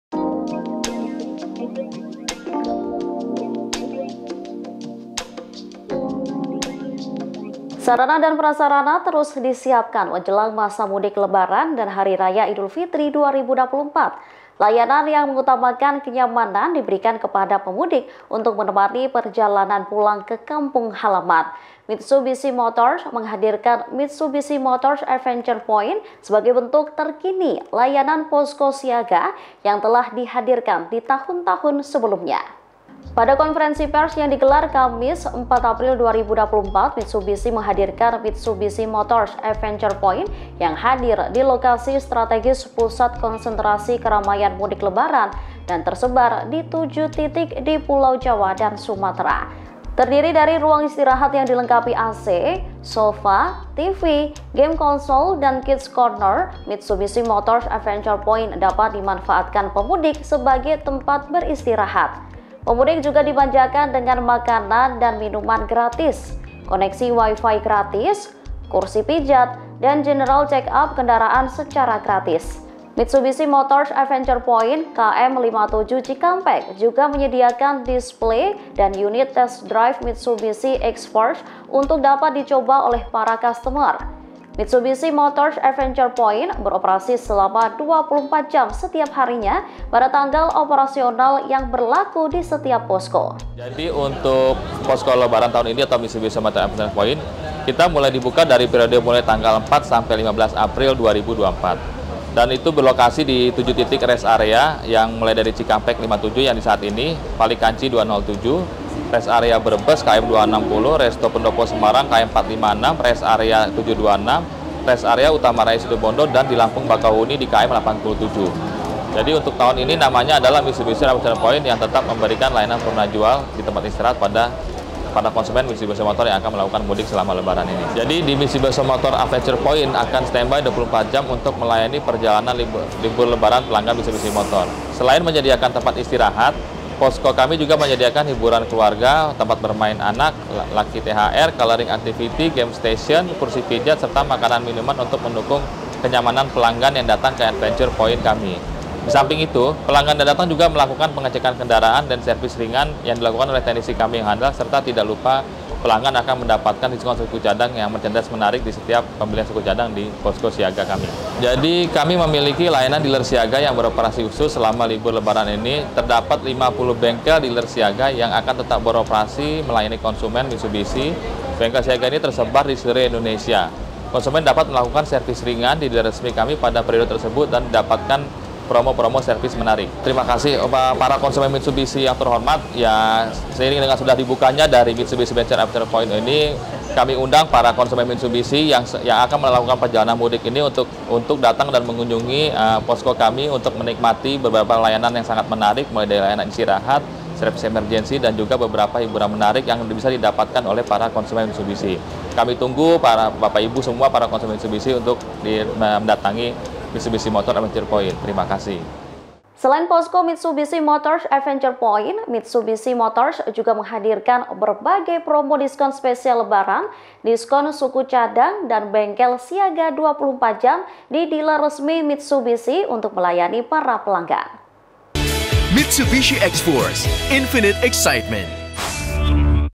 Layanan Posko Siaga bagi Pelanggan, Mistubishi Motors Hadirkan "Mitsubishi Motors Adventure Points"
TRIBUN-VIDEO.COM - Sarana dan prasarana terus disiapkan menjelang masa mudik Lebaran dan Hari Raya Idul Fitri 2024.

Layanan yang mengutamakan kenyamanan diberikan kepada pemudik untuk menemani perjalanan pulang ke kampung halaman.

Mitsubishi Motors menghadirkan “Mitsubishi Motors Adventure Points” sebagai bentuk terkini layanan posko siaga yang telah dihadirkan di tahun-tahun sebelumnya.

Pada Konferensi pers yang digelar Kamis (4/4/2024), Mitsubishi menghadirkan “Mitsubishi Motors Adventure Points” yang hadir di lokasi strategis pusat konsentrasi keramaian mudik Lebaran dan tersebar di 7 titik di Pulau Jawa dan Sumatera.

“Mitsubishi Motors Adventure Points” Tol Cikampek berlokasi di Rest Area –KM 57 A Cikampek berlangsung dari tanggal 4 hingga 13 April 2024.

Terdiri dari ruang istirahat yang dilengkapi AC, sofa, TV, game-console dan kids-corner, Mitsubishi Motors Adventure Points dapat dimanfaatkan pemudik sebagai tempat beristirahat.

Pemudik juga dimanjakan dengan makanan dan minuman gratis, koneksi wifi gratis, kursi pijat, dan general check-up kendaraan secara gratis.

“Mitsubishi Motors Adventure Point” KM 57 Cikampek juga menyediakan display dan unit test drive Mitsubishi Xforce untuk dapat dicoba oleh para customer.

“Mitsubishi Motors Adventure Point” beroperasi selama 24 jam setiap harinya pada tanggal operasional yang berlaku di setiap posko.

Selain posko “Mitsubishi Motors Adventure Point”, Mitsubishi Motors juga menghadirkan berbagai promo diskon spesial Lebaran, diskon suku cadang, dan bengkel siaga 24 jam di dealer resmi Mitsubishi untuk melayani para pelanggan.(*)